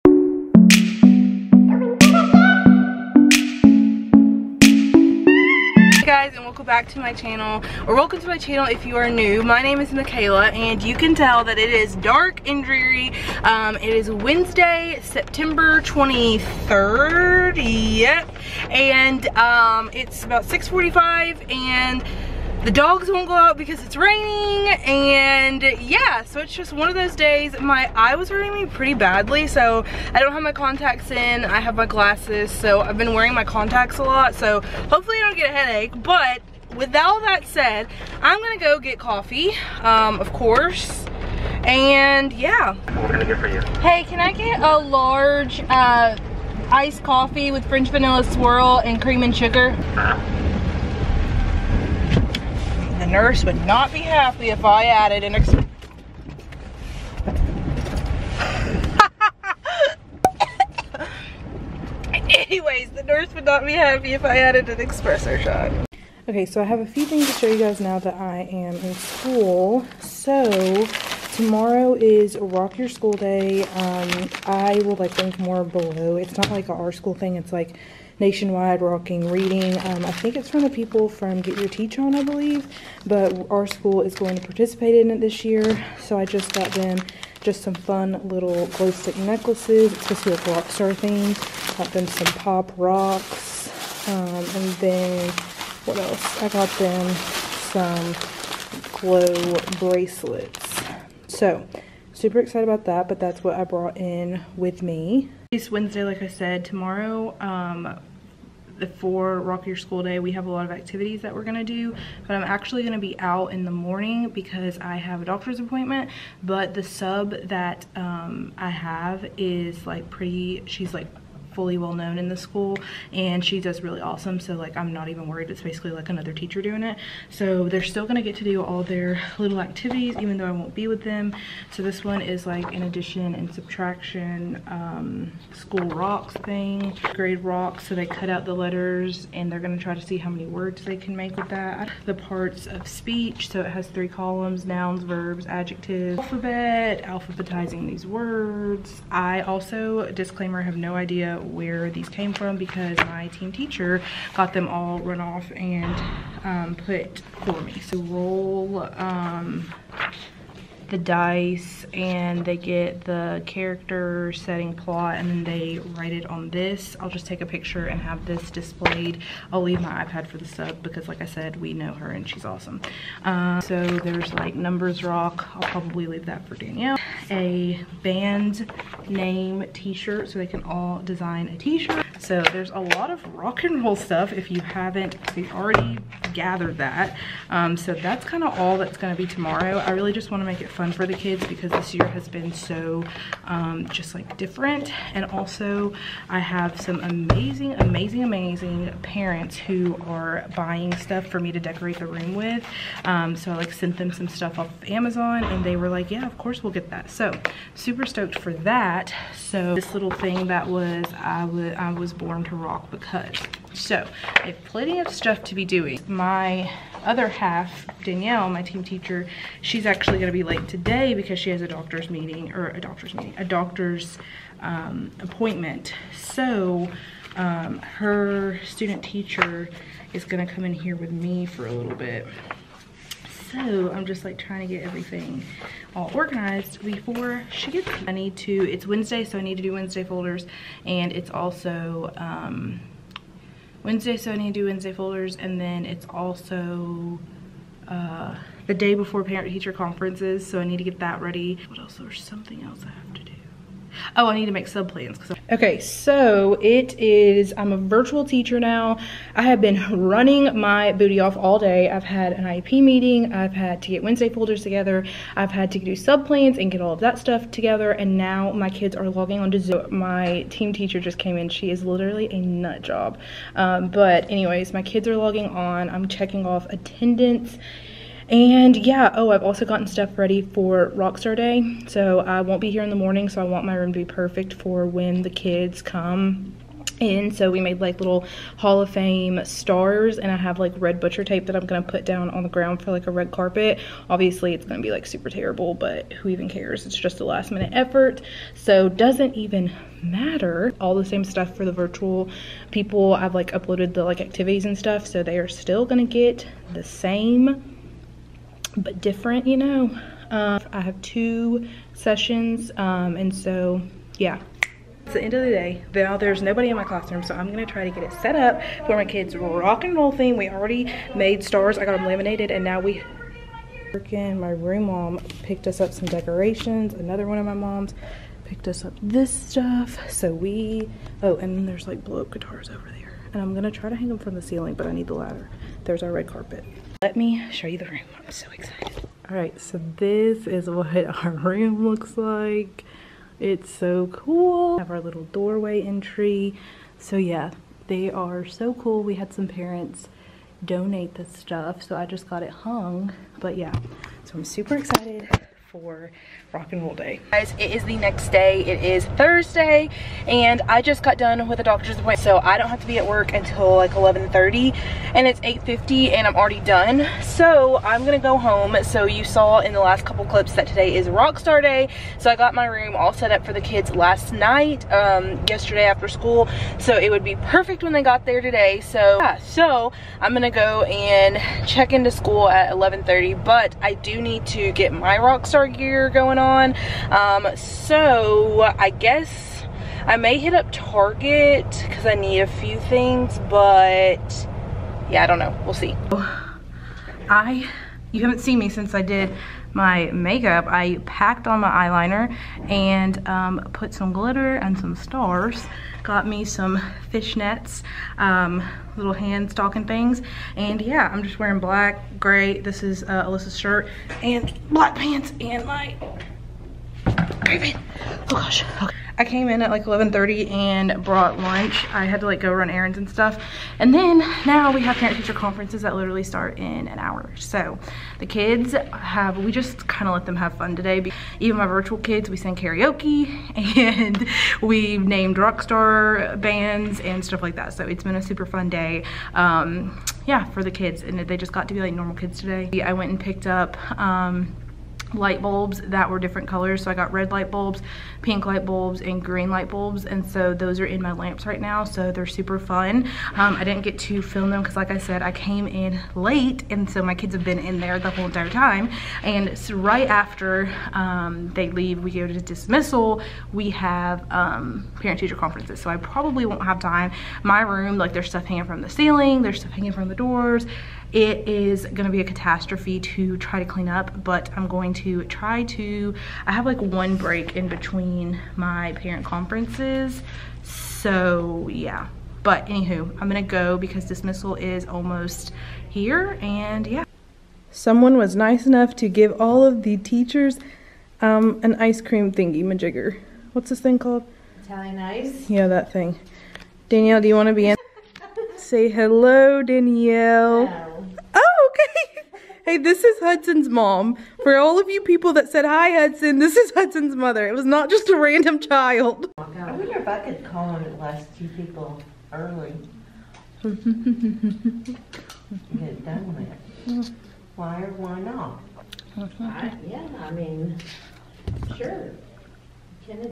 TEACHER VLOG: parent conferences & rock star day!!
✩ SOCIAL MEDIA ✩
teacher insta: @mshodgeshappycampers
vsco: @mckaylapaige

FAQs:
How old are you?
24

Memphis, TN

I'm a 3rd grade teacher